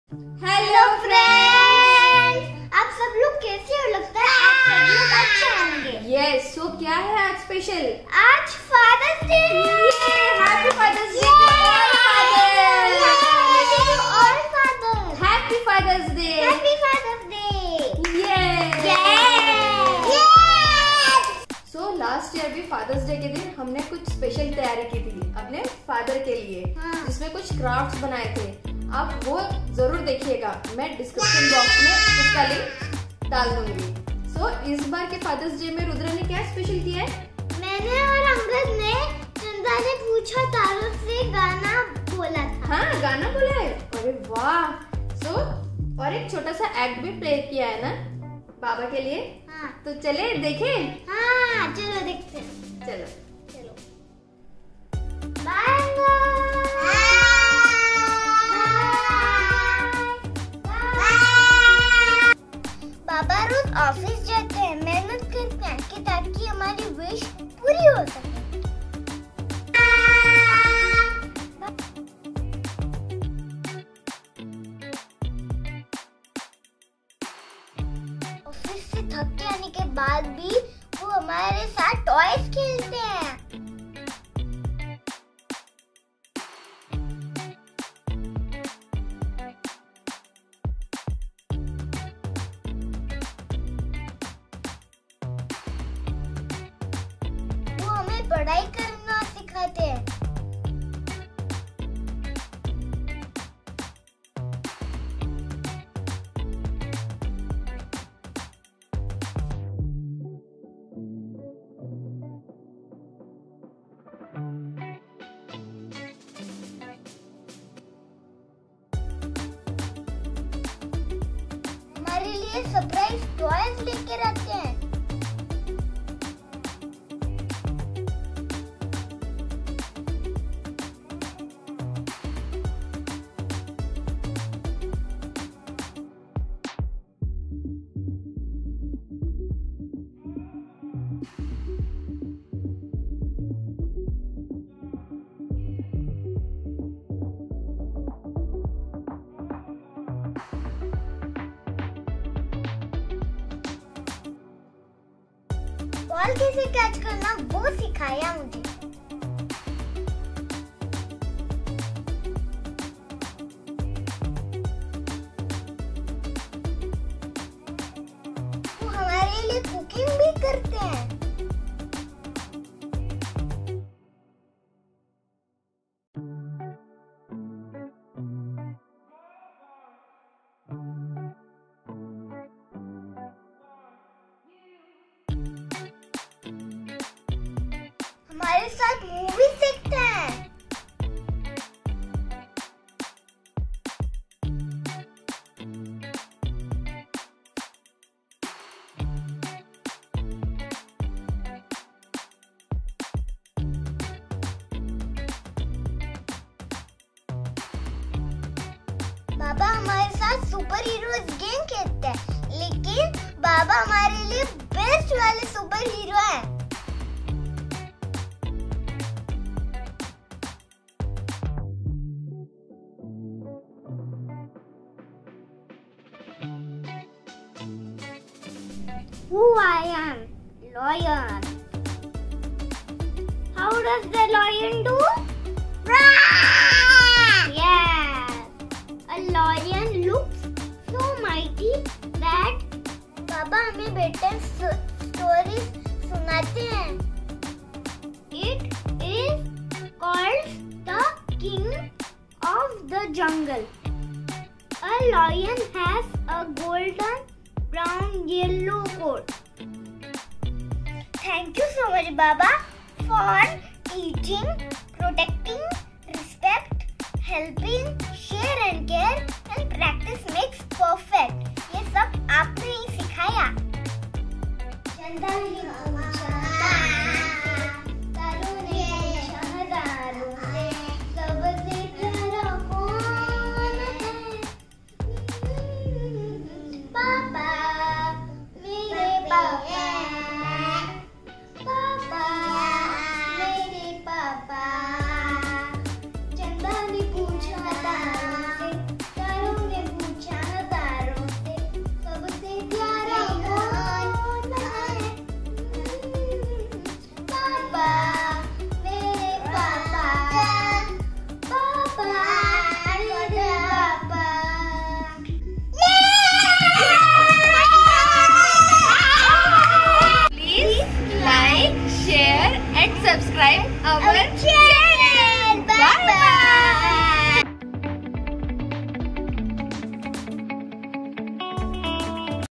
Fathers Day Celebration |Things Fathers Do| Special Act and Song by Kids for Papa on Fathers Day
A small tribute to all the fathers by Rudra and Angad with special act and song on Father's Day.